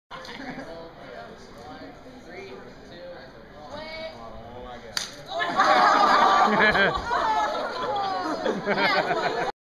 3rd Block Drop 4.MOV
Winter 2011 Eggdrop project drop day video from Sparta High School, Sparta MI